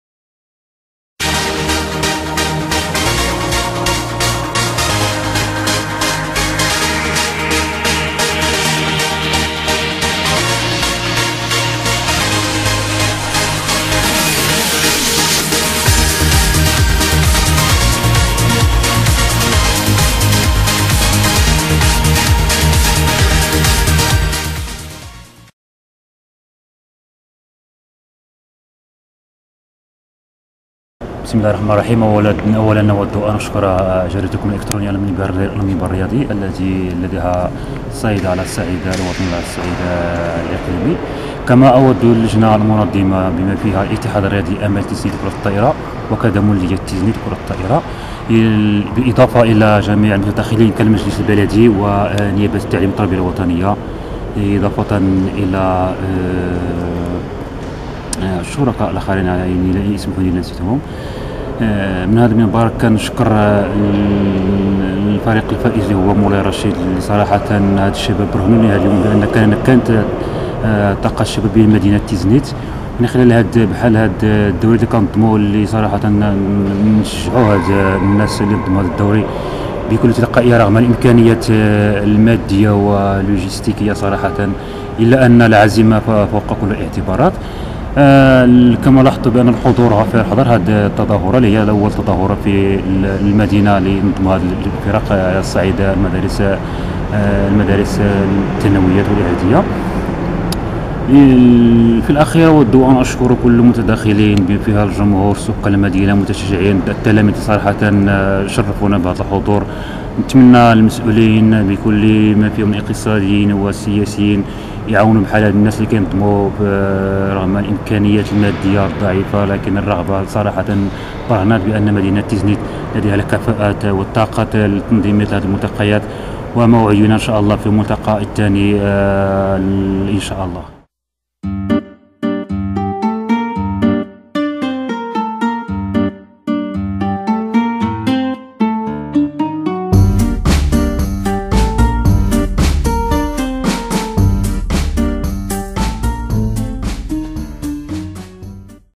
تصريح علي سرحان في اختتام دوري الكرة الطائرة المنظم لفائدة فرق المؤسسات التعليمية 2015/01/17
تصريح علي سرحان عضو المكتب المسير لفريق أمل تيزنيت للكرة الطائرة، بمناسبة الدوري المفتوح المنظم لفائدة تلاميذ المؤسسات التعليمية، بقاعة الرياضات اناروز بمدينة تيزنيت ، بتاريخ 2015/01/17.